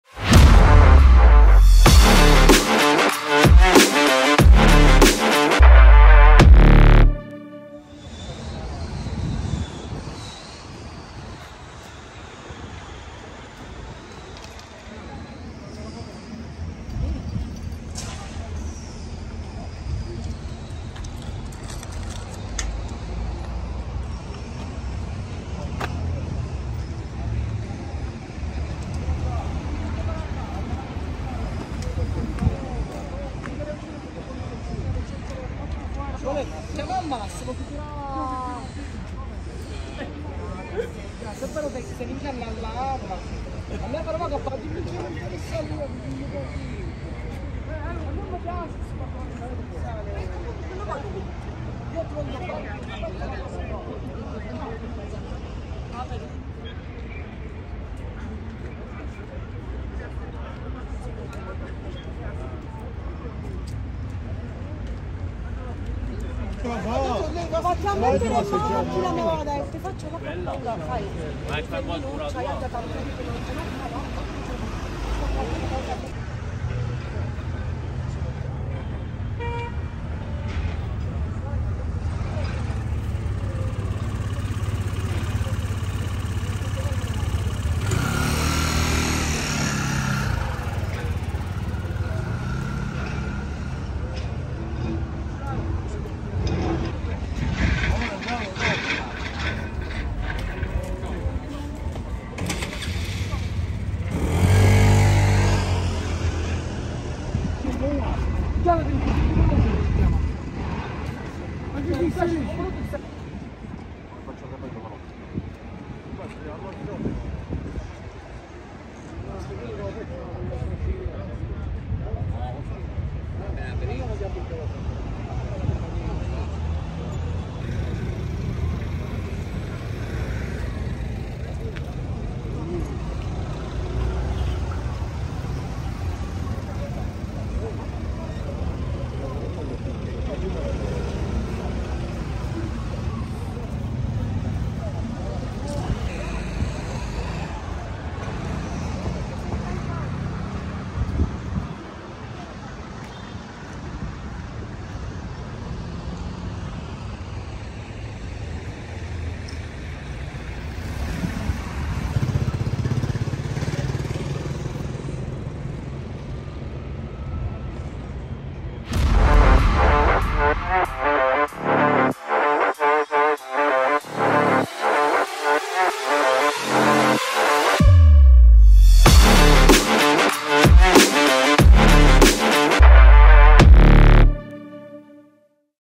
Violento incidente stradale oggi a Palermo
Oggi in Via Archimede all'incrocio con Via della Pace, violento scontro tra un mezzo della RAP ed un furgone.  Gli operatori sanitari intervenuti sul posto hanno trasportato in ospedale l'operaio della RAP che presentava diverse contusioni.